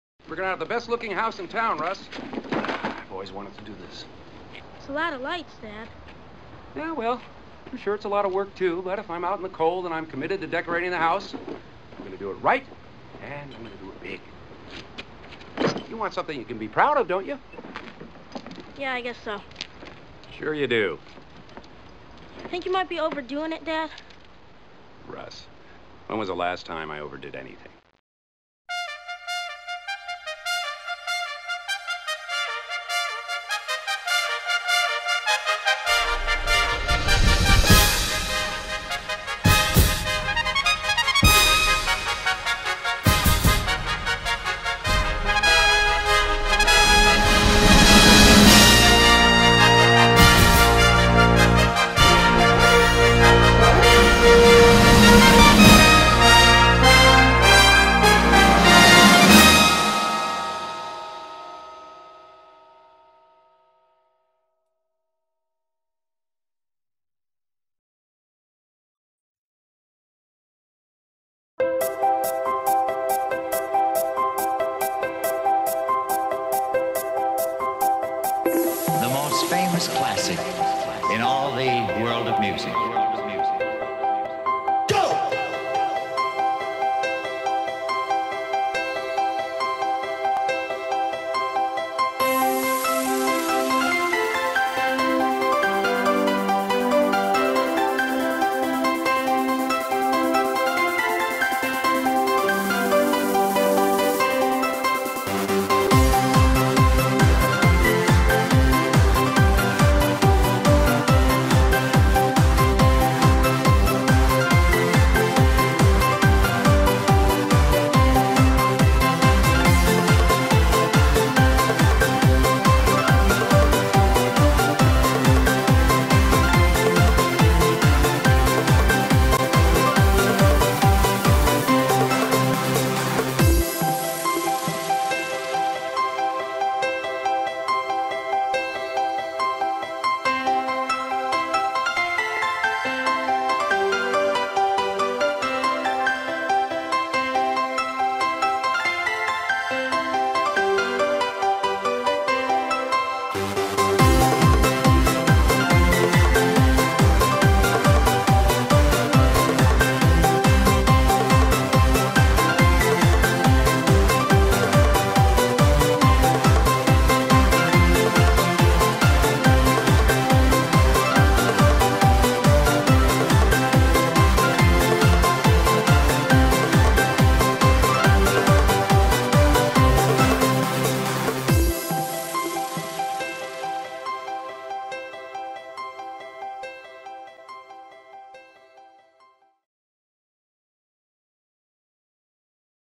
Lights on Stanfield 2014
Our 2014 Light-O-Rama display at 190 Stanfield Ave, Dartmouth
Sunday - Thursday 5 pm - 10 pm
Friday - Saturday 5 pm - 11 pm